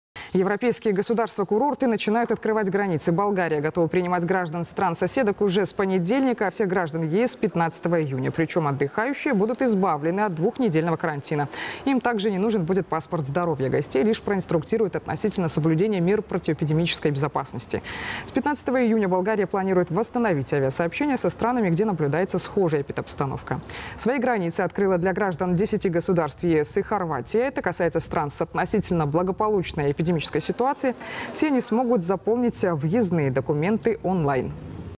Болгария ждёт на своих курортах туристов
Болгария готова принимать граждан стран-соседок уже с 1 июня, а всех граждан ЕС с 15 июня. Причем, отдыхающие будут избавлены от двухнедельного карантина! Им также не нужен будет «паспорт здоровья».